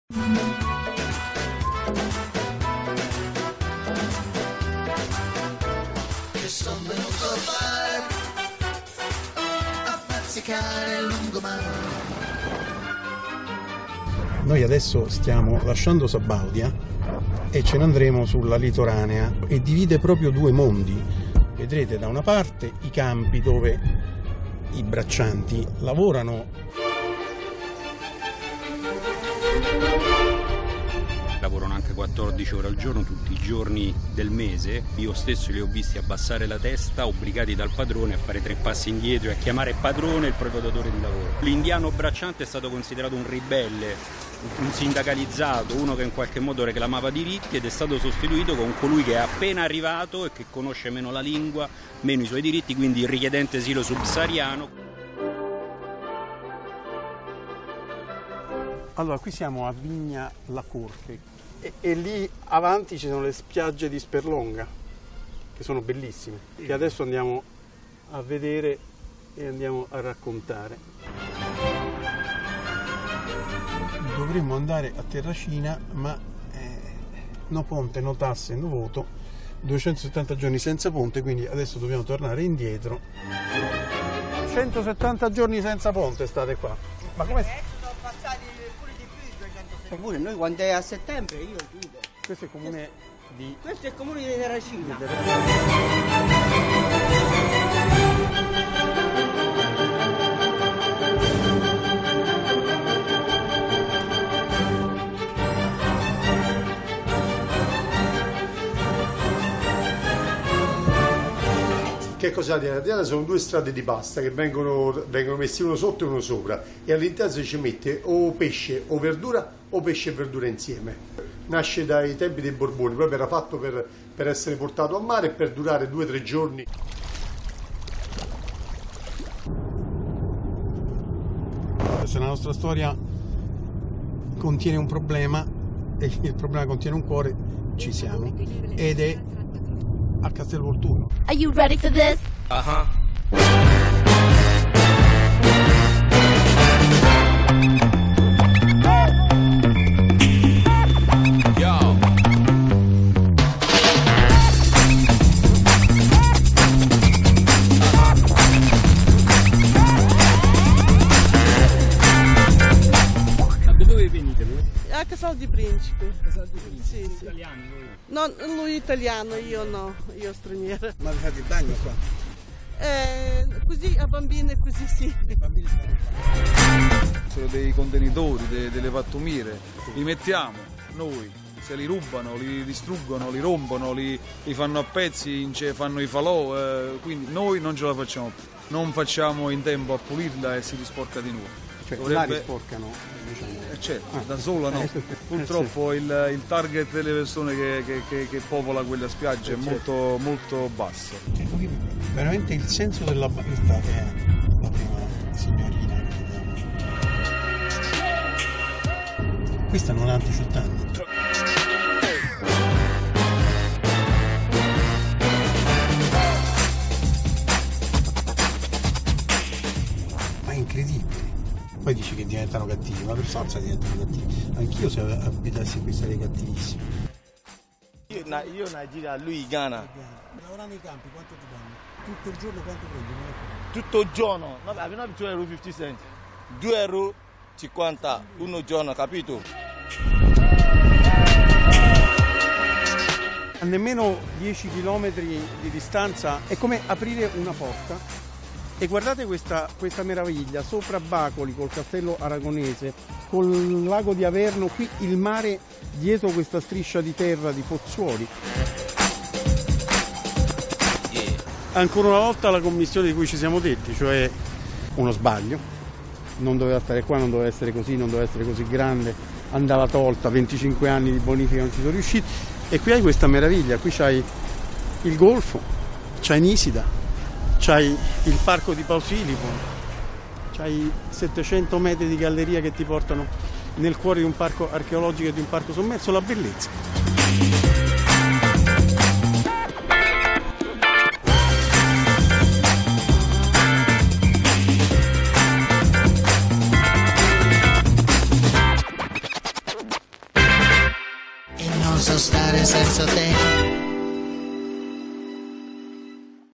Lunghissimo Lungomare - Tappa12 - Tra bellezza e ferocia
La litoranea di San Felice percorsa dai braccianti agricoli e la triste archeologia industriale di Bagnoli. In mezzo ville romane, ponti abbattuti e mai ricostruiti, chioschi trasformati in luoghi del delitto dalla camorra. Ma la bellezza deve vincere, anche qui. Parola di un fioraio sorridente
di Goffredo Buccini - video di Antonio Crispino